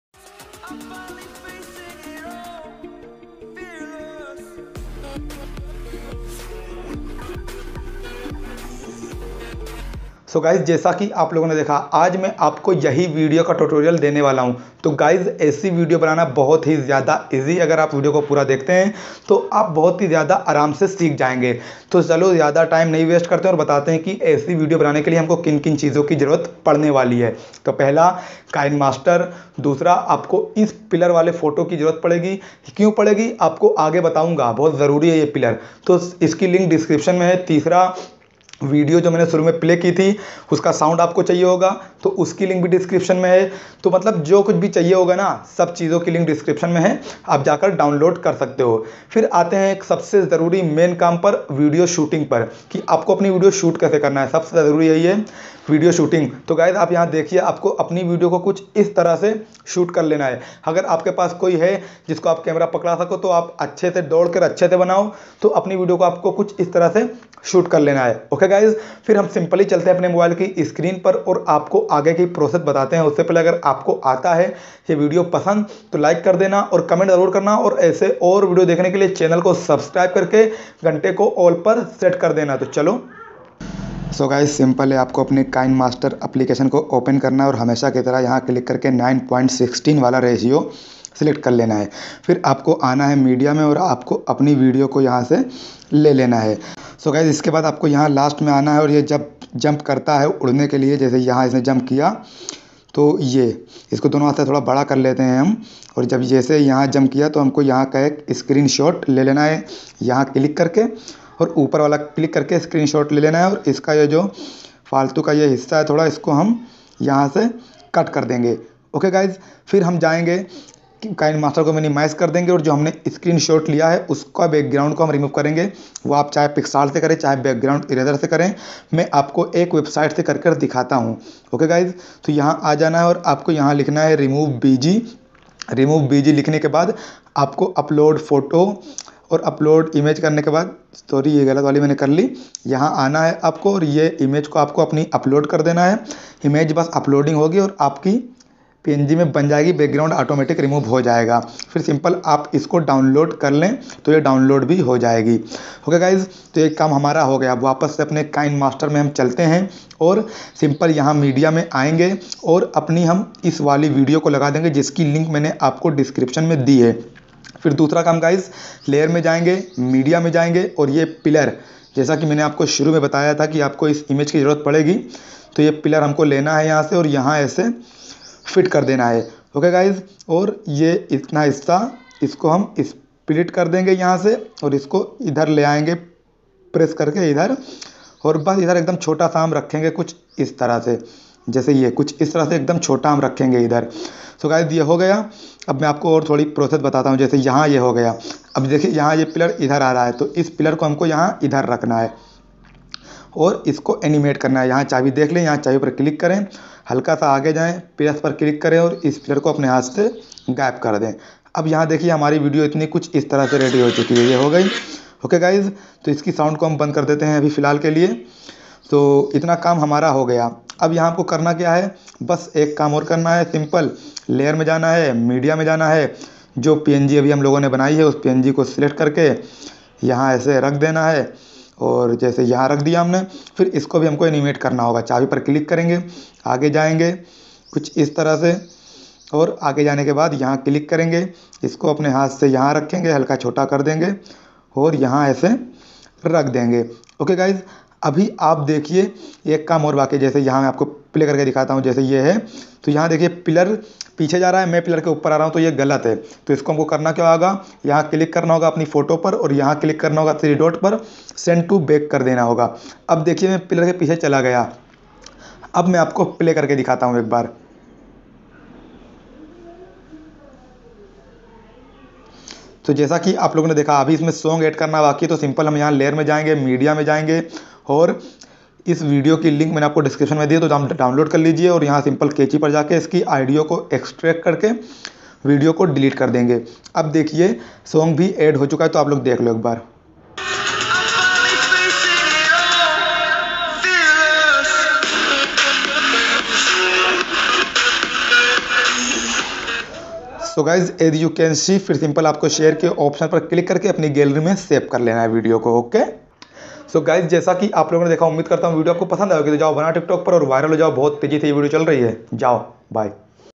TikTok Video Editing Tutorial | Fly In The Sky Transition Video Editing | Kinemaster Video Editing
TikTok Video Editing Tutorial | Fly In The Sky Transition Video Editing | Asman Me Udne Wali Video Kaise Banaye | Kinemaster Video Editing

DOSTO AJ ME APKE LIYE LEKE AYA HUN EK AUR MAZEDAR VIDEO SO GYS AJ ME APKO BATANE WALE HUN TIKTOK KI NEW VIRAL VIDEO EDITING JO KI FLY IN THE SKY TRANSITION TUTORIAL HAI APLOGO NE DEKHA HOGA BAHUT VIRAL HO RHI HAI TO CHALO APKO BHI SIKHATE HAI
OR DOSTO AGAR VIDEO PASAND ATA HAI TO VIDEO KO
( LIKE OR SHARE ) JARUR KARNA
OR AGAR HO SAKE TO
CHANNEL KO
KAR LENA BYE

Piller photo

Sky video

Tiktok sound

Kinemaster